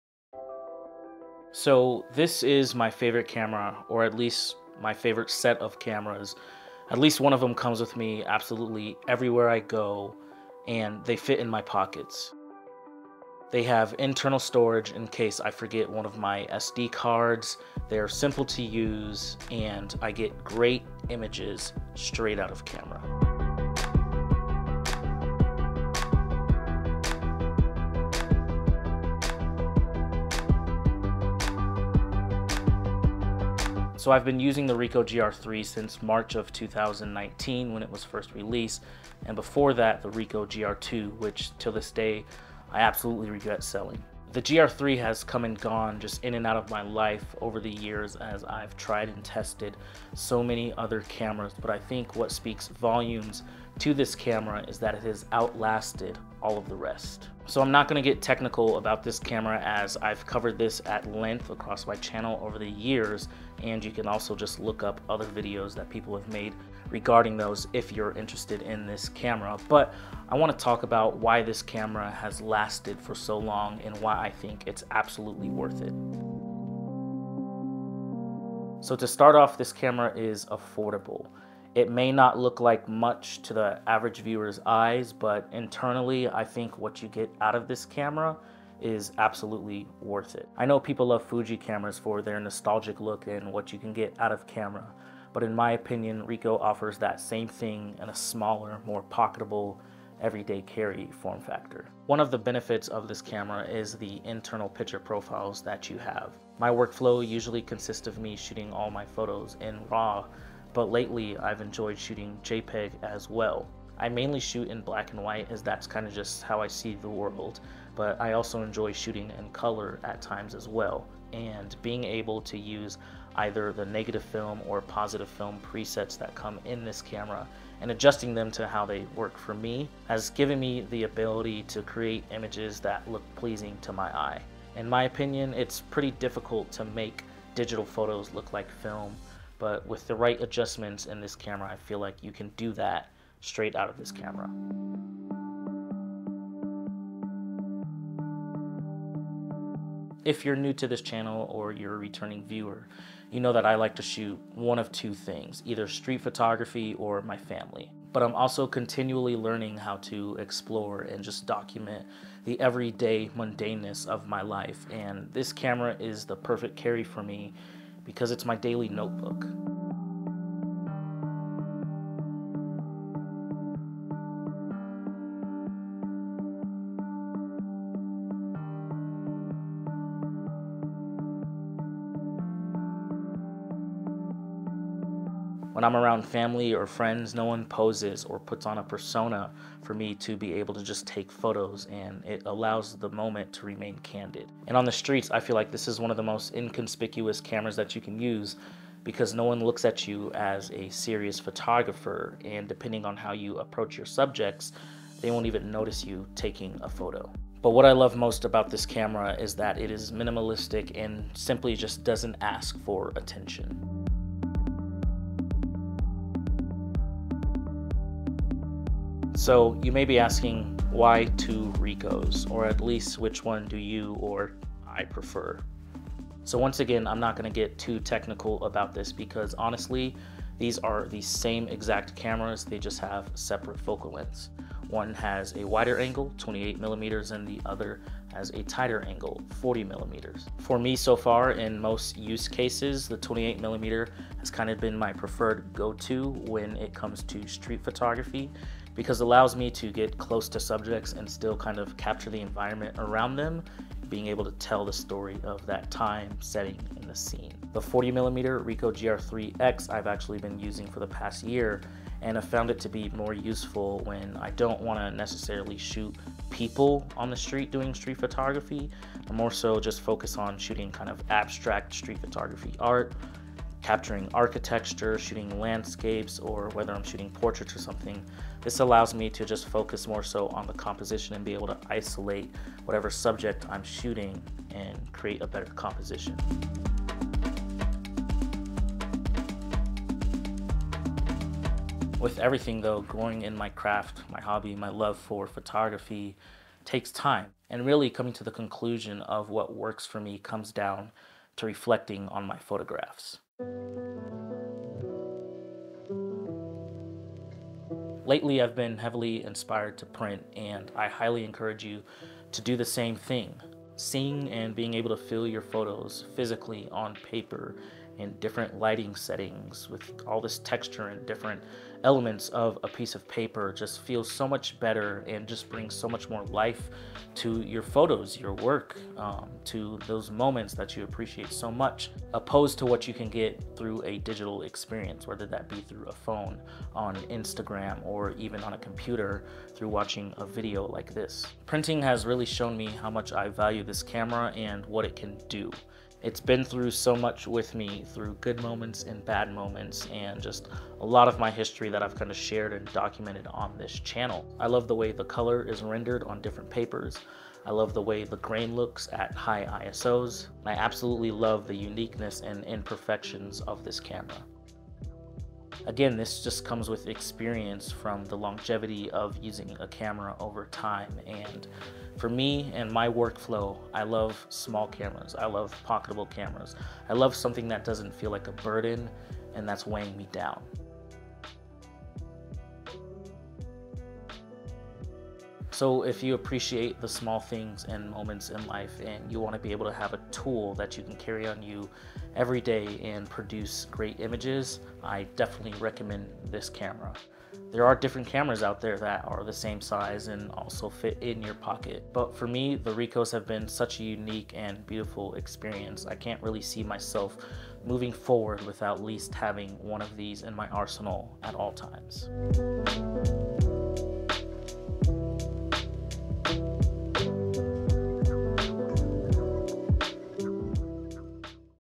smallest film like photo camera you should own
When it comes to a small film-like photo camera. The Ricoh Grii and Griiix are two of the best and smallest most versatile film-like photo camera you should own. They are affordable compared to what most people love nowadays such as the Fujifilm line of cameras and more importantly the fujix100v. But this camera is one that you will carry in your pocket to take photos everywhere you go.

Ricoh griii
Ricoh Griii x